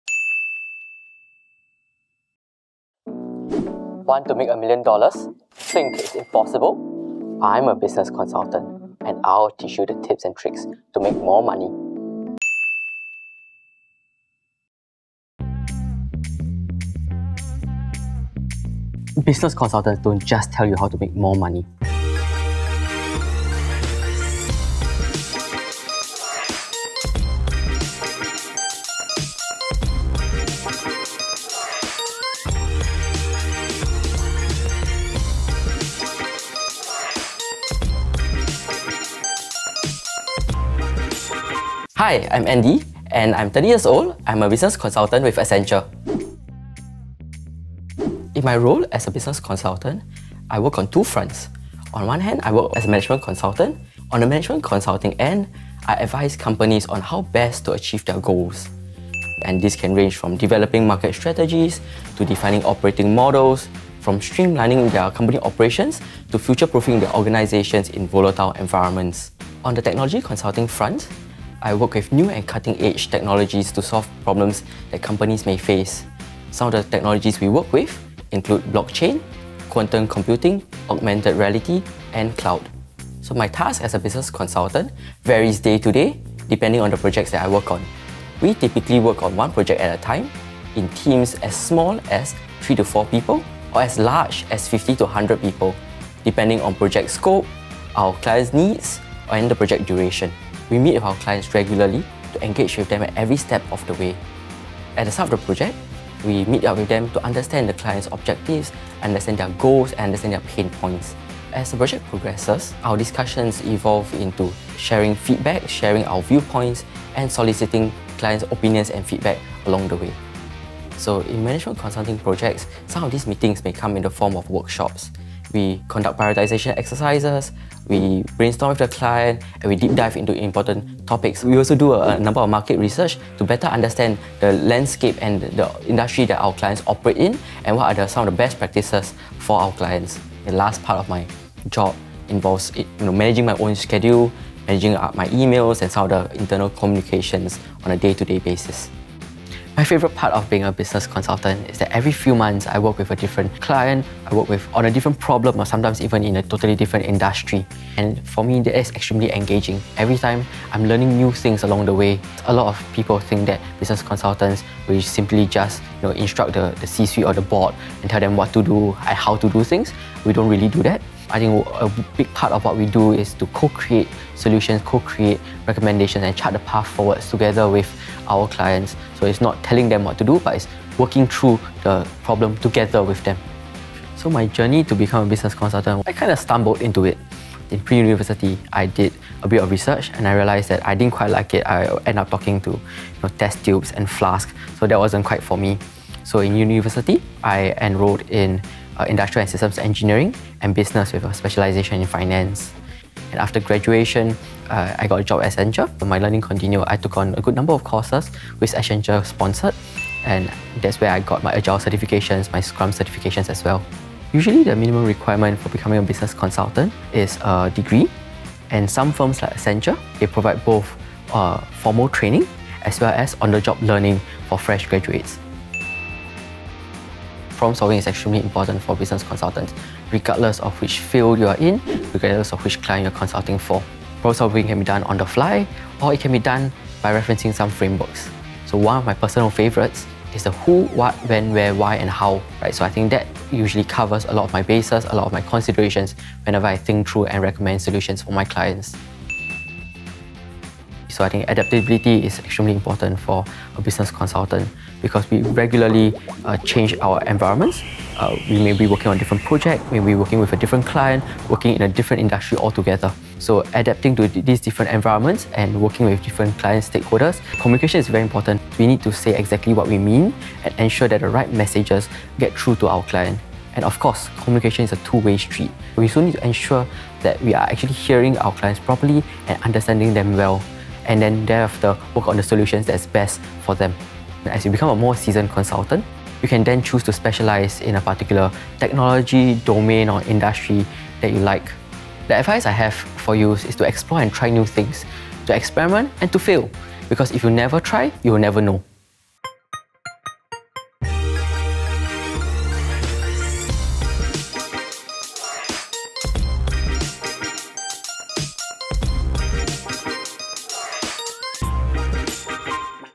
On My Way: A Day in the Life of a Business Consultant
Business consultants don't just tell you how to make money - they deep dive to examine many aspects of a business such as its structures, processes, and teams! Let Andy tell you what business consultants actually do in this video.

Explore information about careers and discover resources at On My Way!